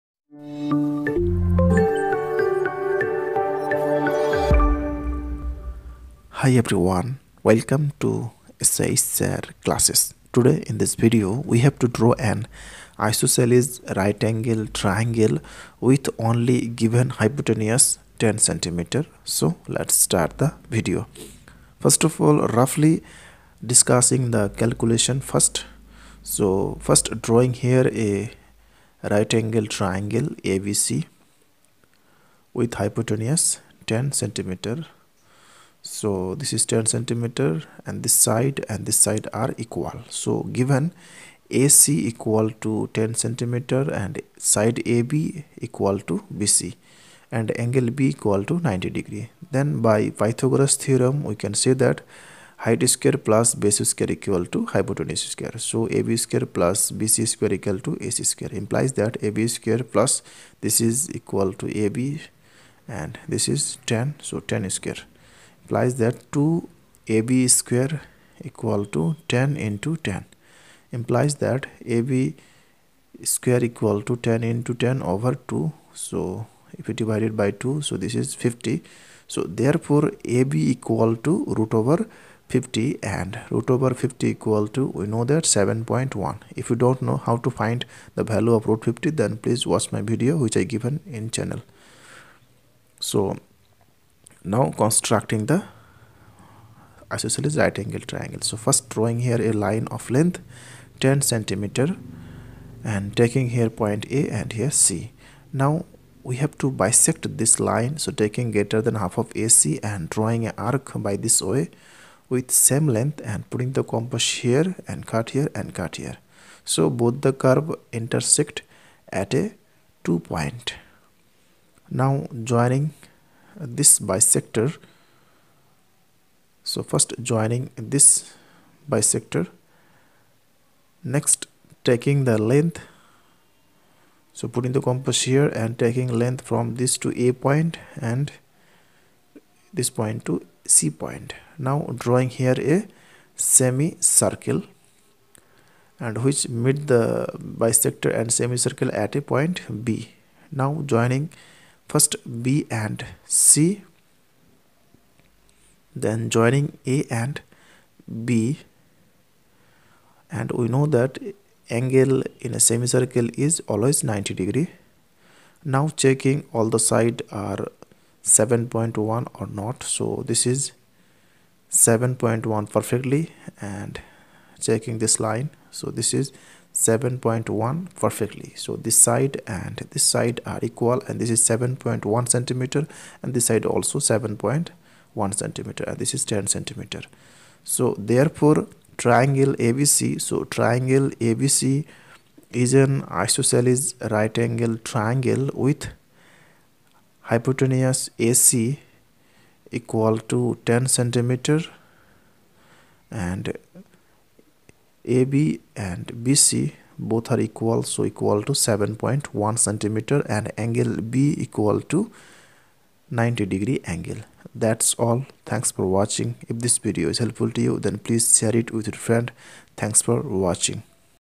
How to construct an isosceles right angle triangle whose hypotenuse 10cm.shsirclasses.
In Easy Way how to construct an isosceles right angle triangle whose hypotenuse 10cm.shsirclasses. Geometrical Construction.